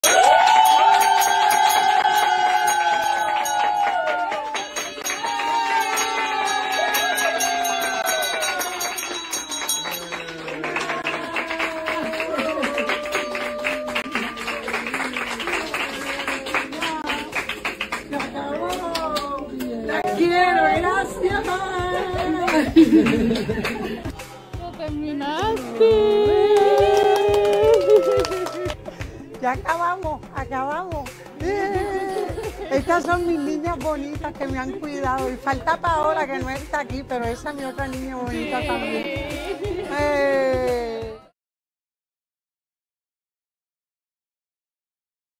Marian Pabón revela algo importante de su salud en emotivo video
La actriz no pudo contener la emoción y conmovió a muchos en las redes.